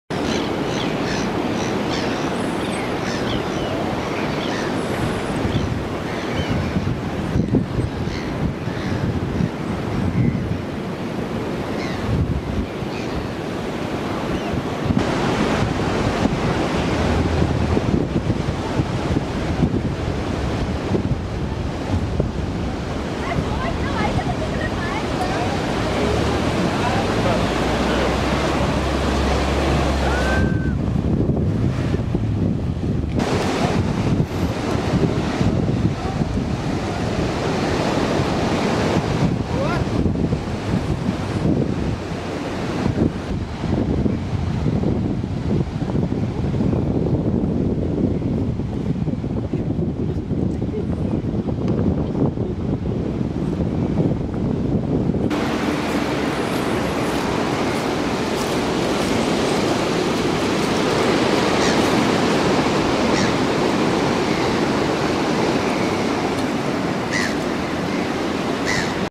Cape Town 2013 - Sea Point
Wenn es woanders Stuermt, in Sea Point ist es meistens windstill.